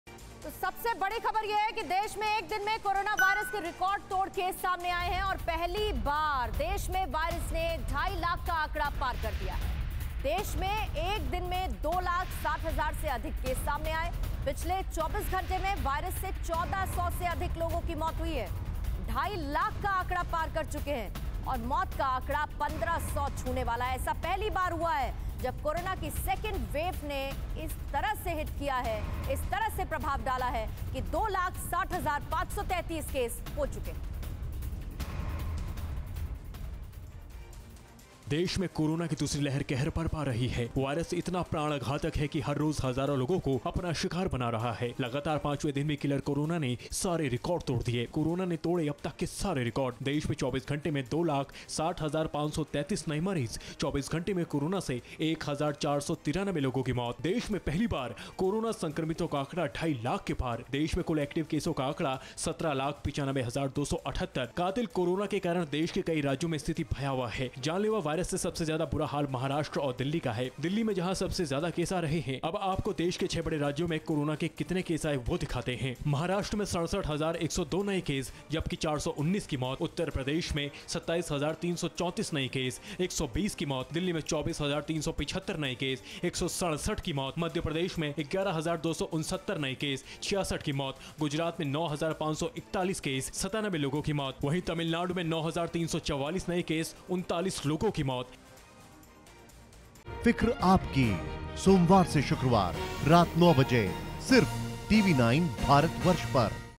Corona Alert : देश में एक दिन में कोरोना के रिकॉर्ड तोड़ केस आए सामने
देश में एक दिन में कोरोना वायरस के रिकॉर्ड तोड़ केस सामने आए हैं. पहली बार देश में वायरस ने ढाई लाख का आंकड़ा पार किया है. देश में एक दिन में 2 लाख 60 हजार से अधिक केस सामने आए हैं. पिछले 24 घंटे में वायरस से 1400 से अधिक लोगों की मौत हुई है.

Credit